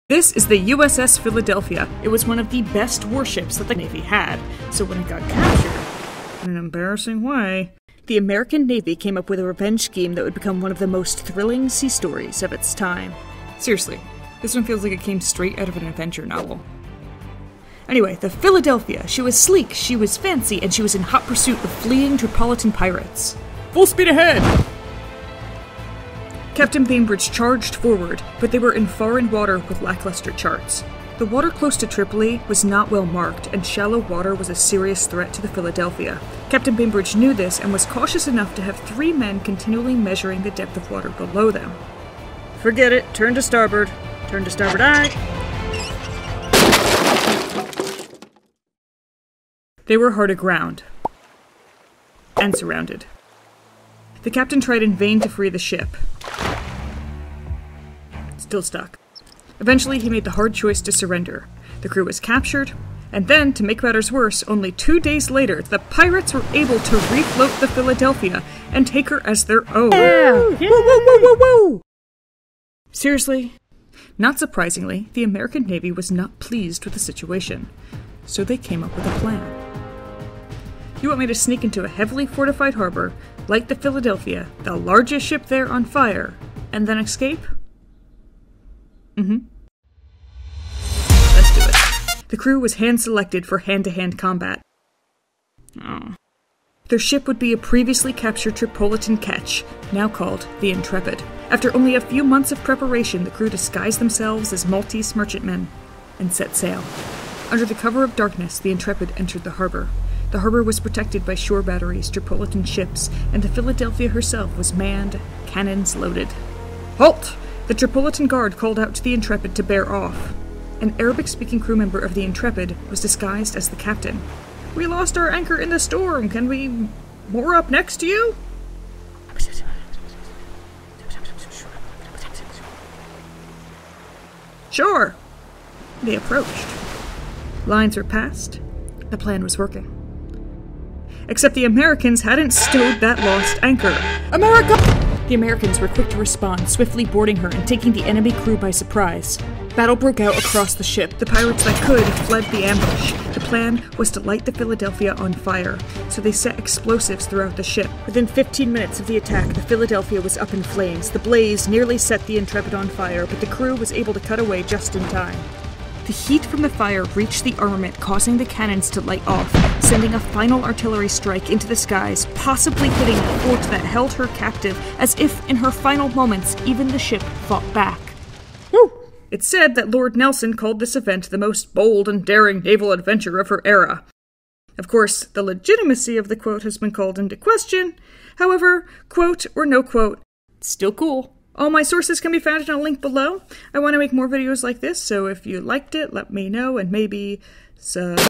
The INSANE REVENGE Story to DESTROY The USS Philadelphia | Animated History
The USS Philadelphia was one of the US Navy’s best warships, but it ran aground and was captured by Tripolitan Pirates during the Barbary Wars. The US Navy couldn't stand for that so they hatched an insane revenge scheme. Stephen Decatur and a disguised crew sailed into Tripoli Harbor to light the USS Philadelphia on fire. If the United States couldn't have the ship - no one could.

The History of the USS Philadelphia is one of History's Great Adventure stories.

ANIMATORS:
Demi Bozas
Myself

SOURCES:

(^^^ love this book )